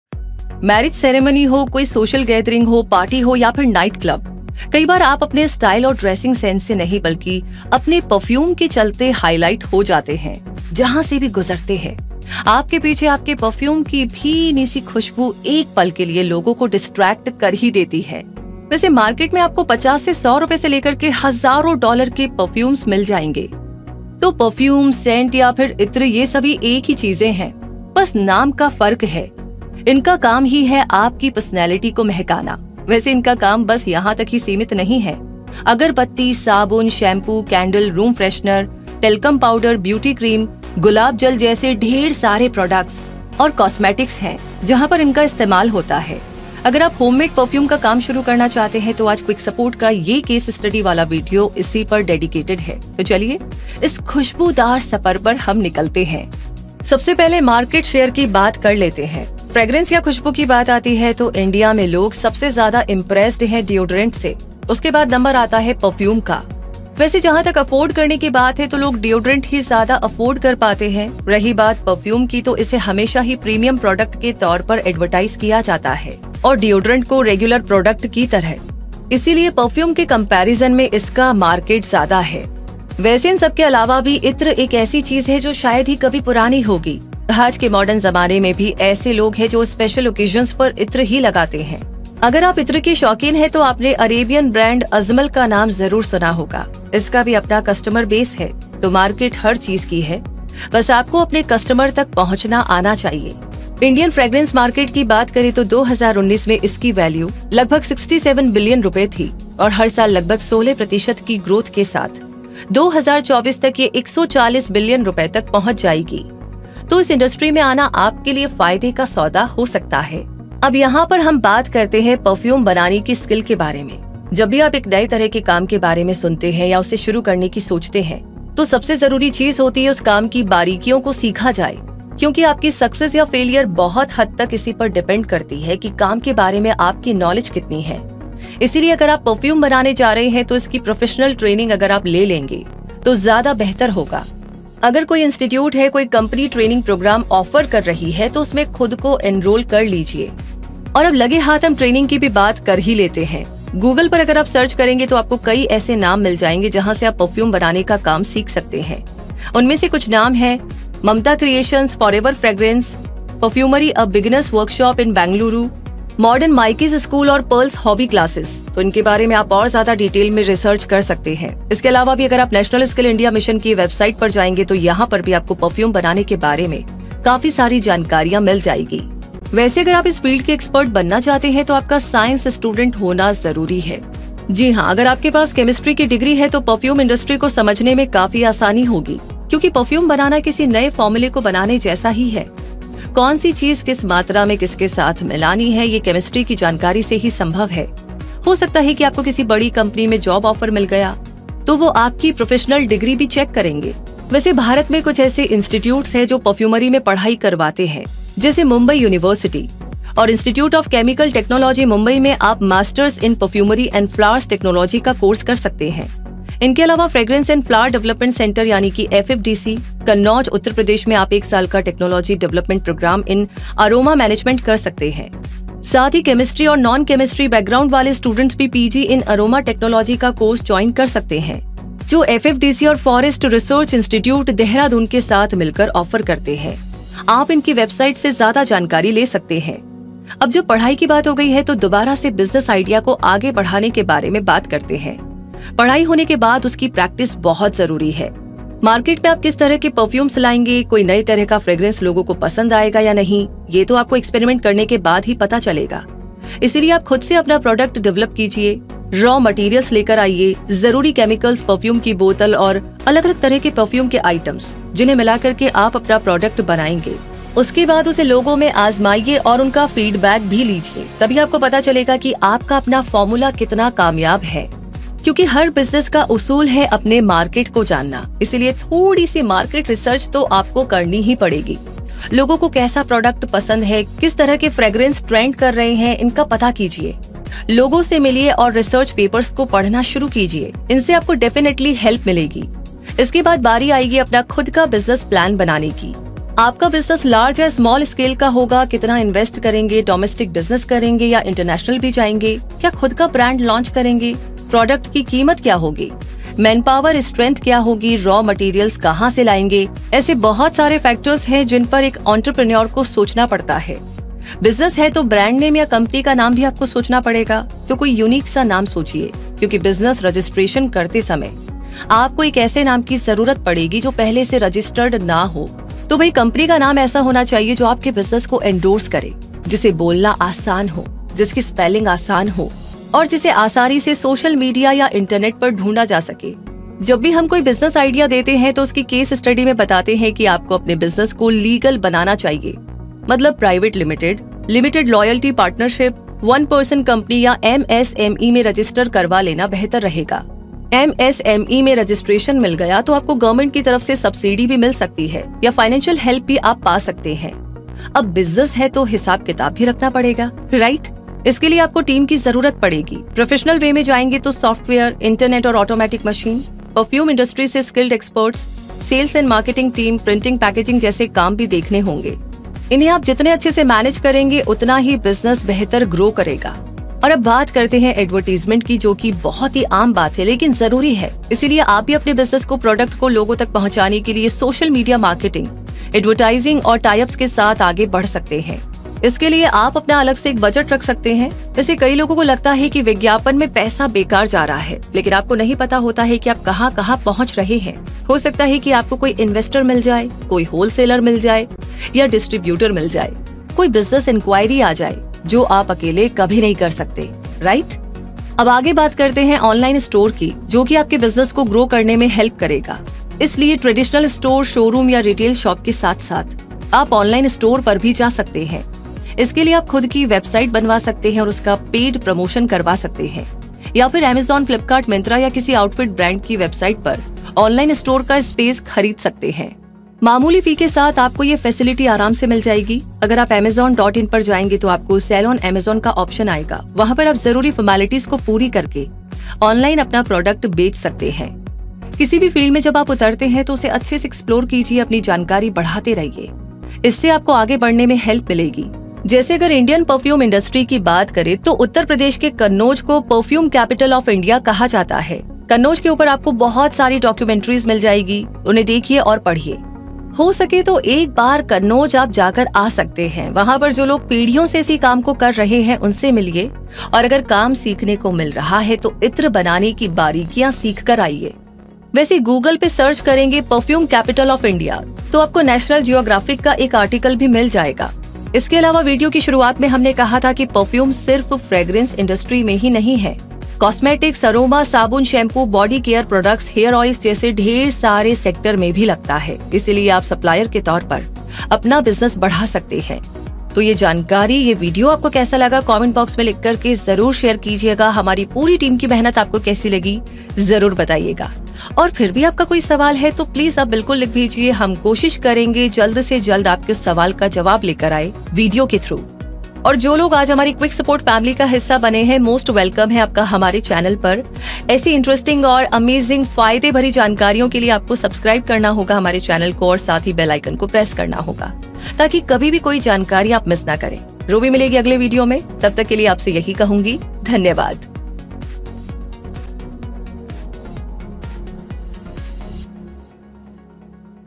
How to Start Homemade Perfume Business With Full Case Study? – [Hindi] – Quick Support
HowtoStartHomemadePerfumeBusiness?

How to Start Homemade Perfume Business With Full Case Study? – [Hindi] – Quick Support. अगर आप Homemade perfume का काम शुरू करना चाहते हैं, तो आज Quick Support का ये Case Study वाला वीडियो इसी पे Dedicated है. इसीलिए इस विडियो को पूरा जरुर देखे.